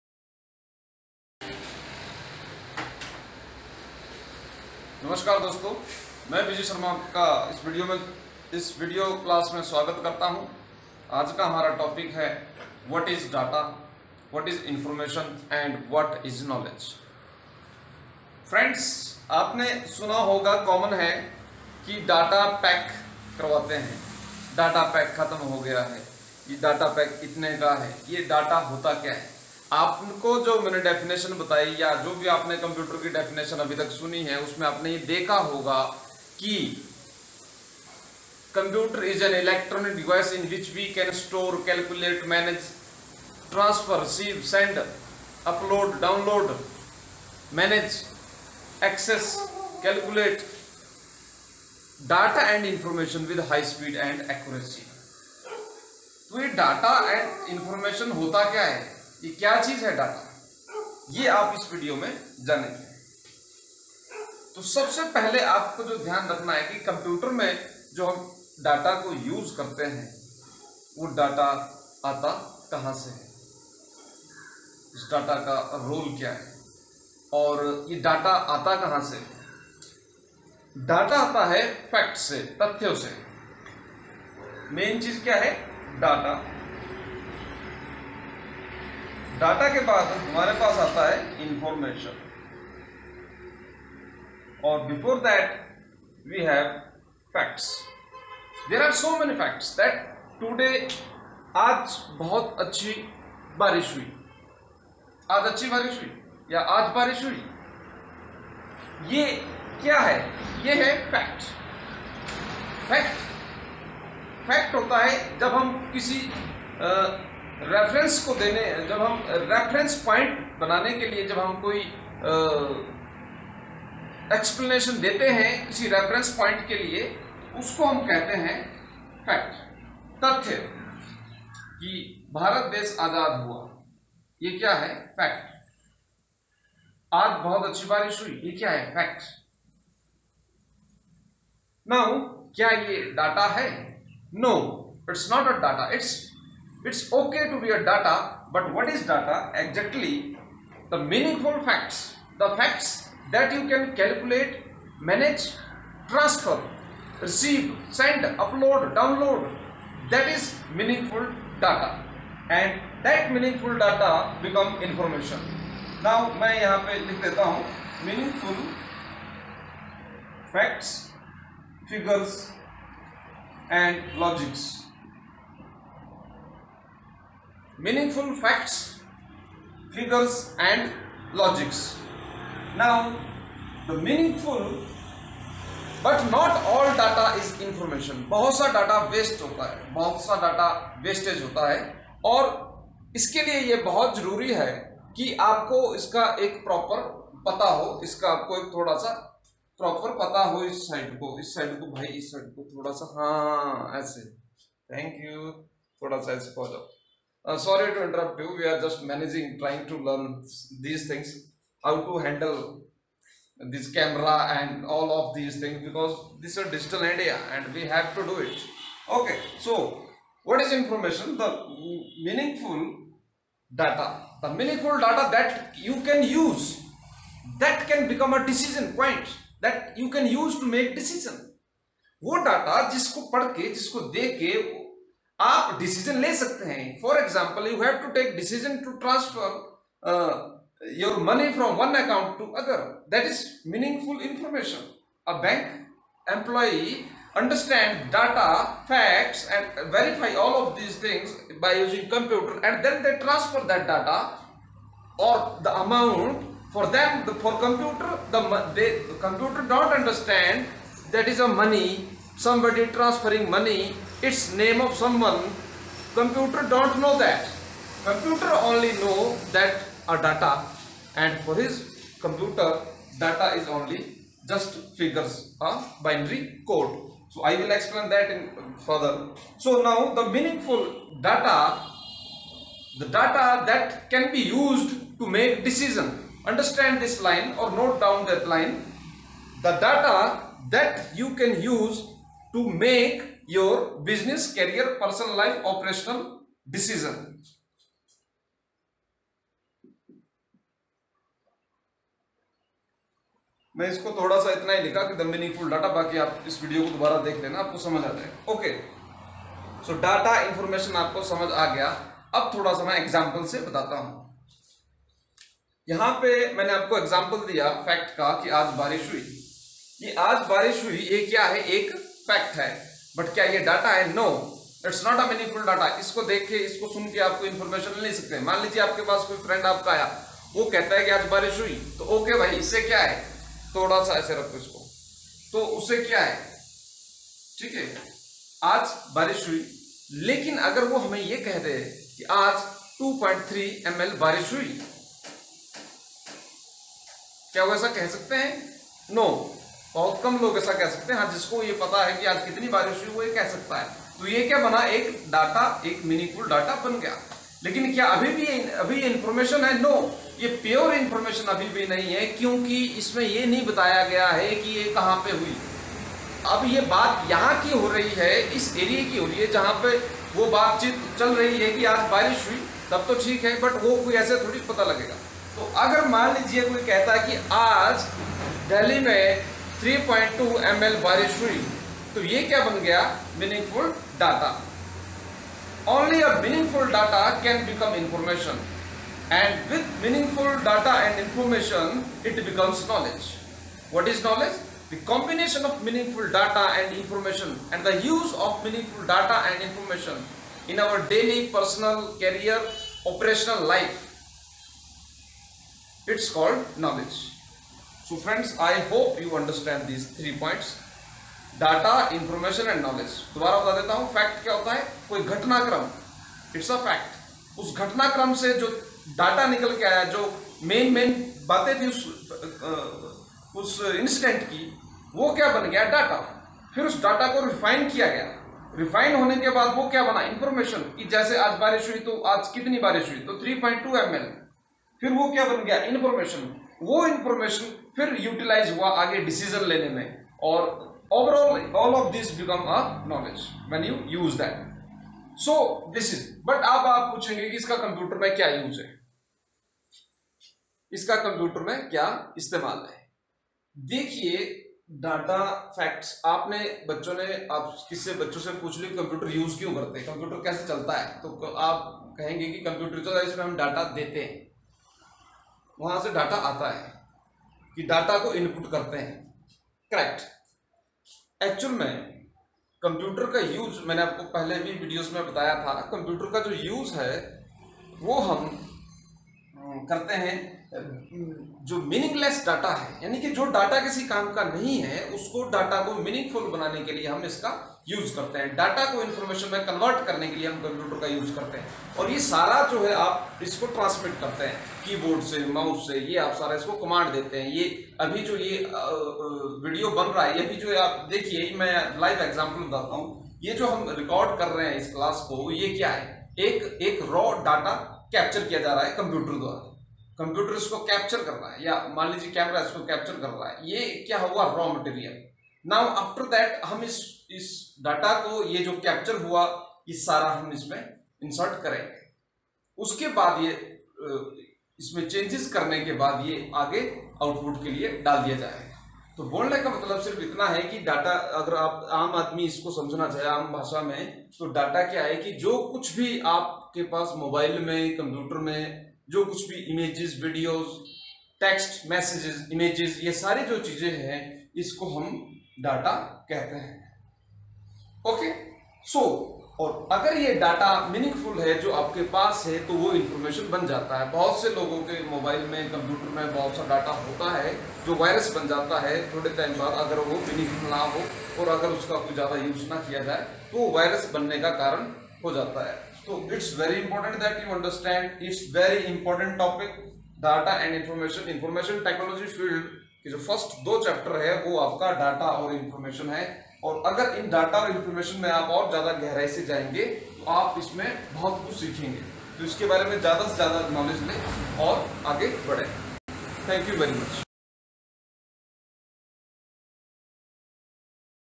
Online Basic Computer Course: Data and Information - Class 0004 by Vijay Sharma
In this video class, you can watch and learn about what is data, information, and how it works and why it's important. The language of this class is Hindi and English. The explanations are personal and based on observations and own learning experiences.